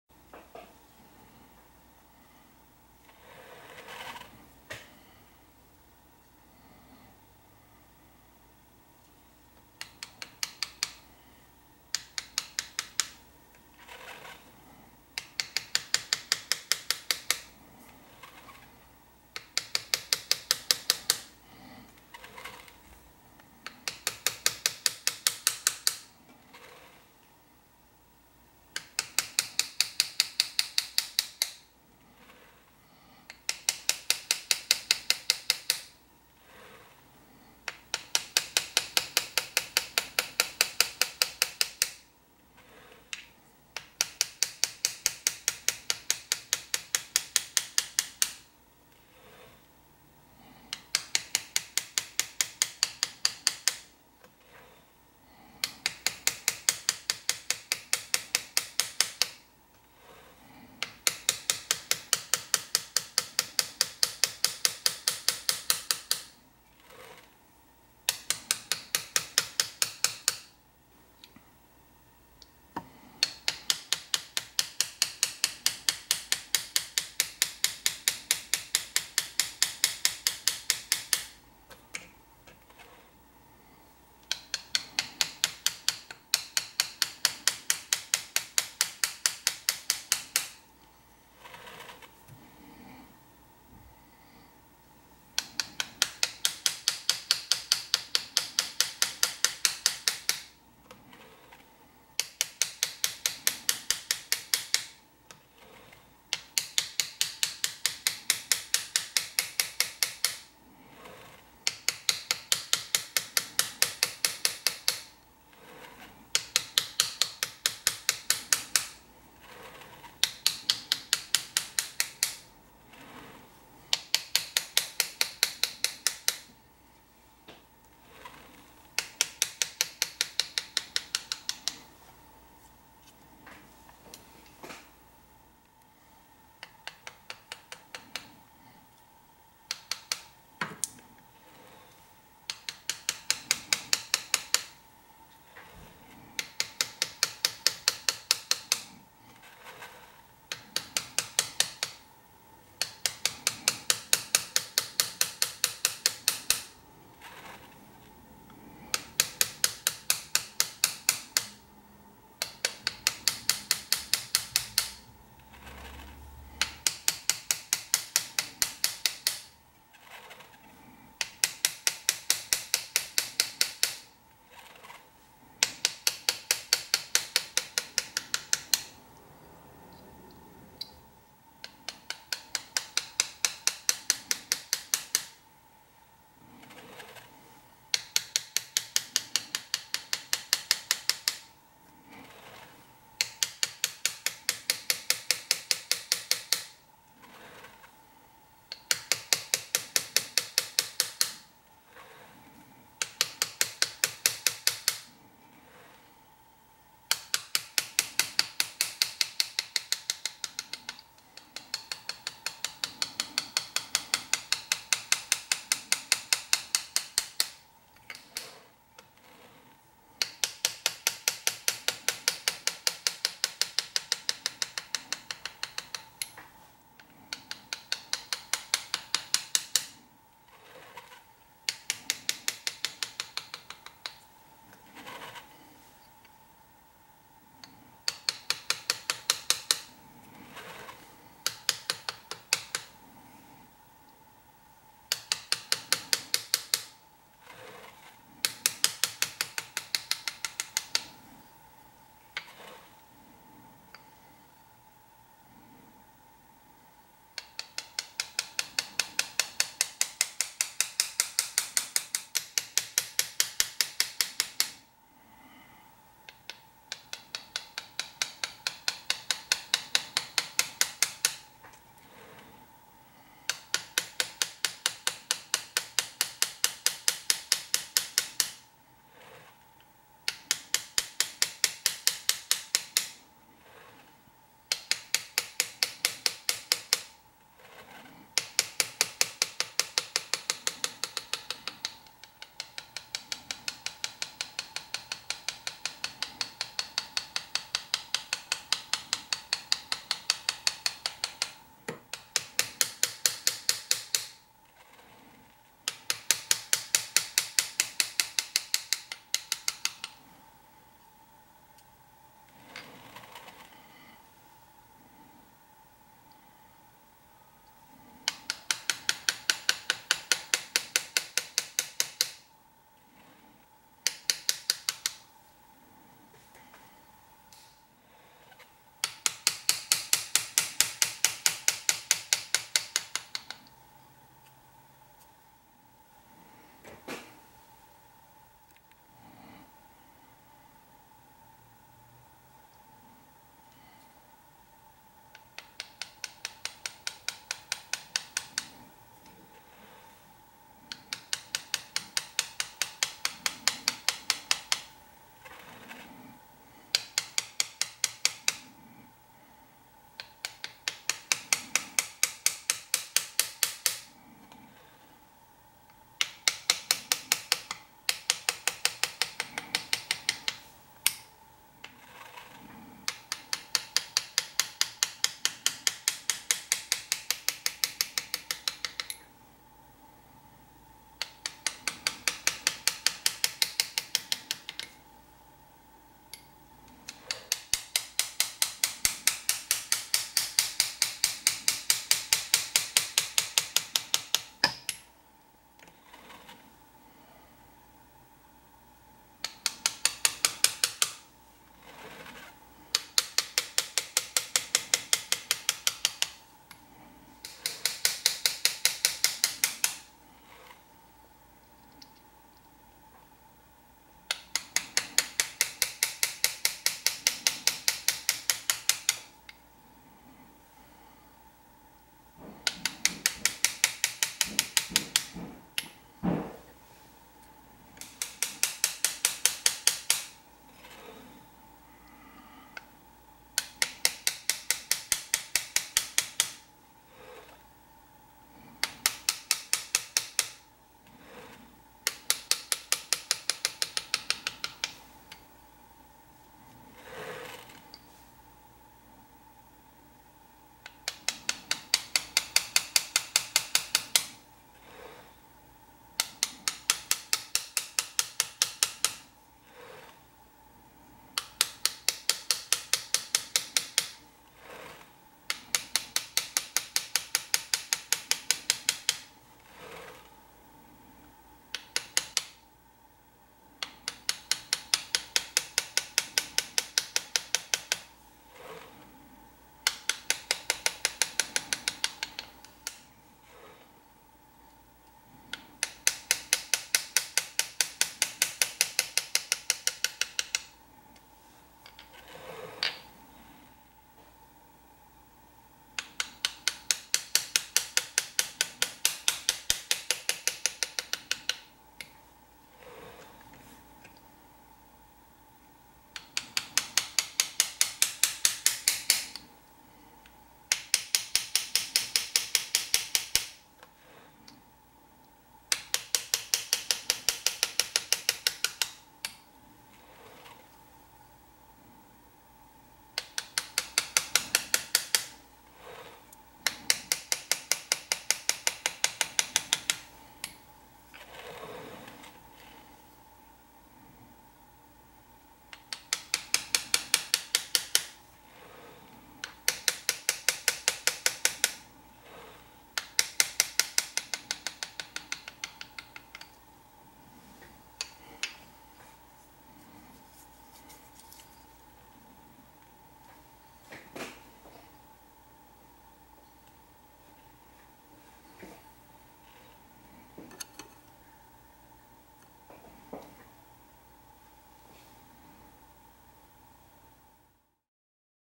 Hammer & Chisel 2016 demonstration by Sam Alfano
Hammer & chisel engraving while sitting using a stereo microscope.